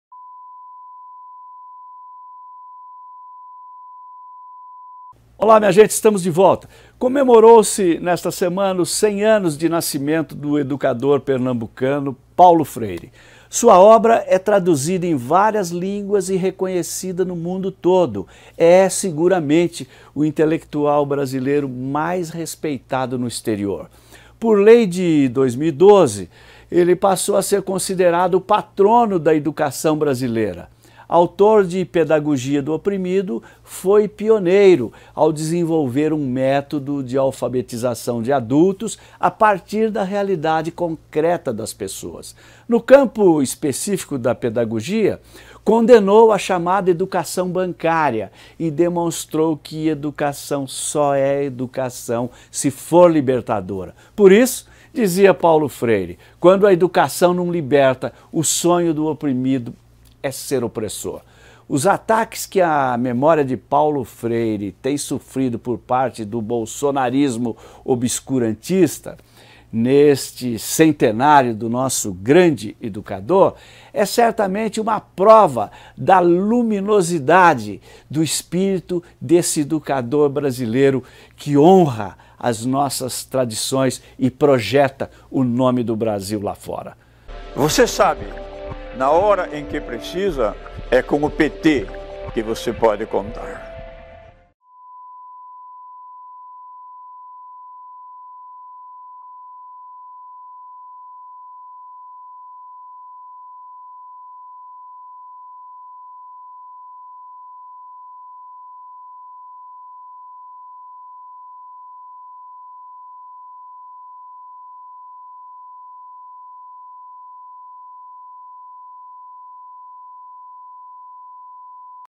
machadopodcast | 100 ANOS DE PAULO FREIRE
COMEMOROU-SE nesta semana os 100 anos de nascimento do educador pernambucano Paulo Freire. Sua obra é traduzida em várias línguas e reconhecida no mundo todo. É o intelectual brasileiro mais respeitado no exterior.

Por lei de 2012, passou a ser considerado o Patrono da Educação Brasileira. Autor de "Pedagogia do oprimido", foi pioneiro ao desenvolver um método de alfabetização de adultos a partir da realidade concreta das pessoas.

No campo da pedagogia, condenou a "educação bancária" e demonstrou que Educação só é Educação se for libertadora; por isso, dizia: "Quando a Educação não liberta, o sonho do oprimido é ser opressor".

Os ataques que a memória de Paulo Freire tem sofrido por parte do bolsonarismo obscurantista, neste seu centenário, é certamente mais uma prova da luminosidade do grande educador brasileiro.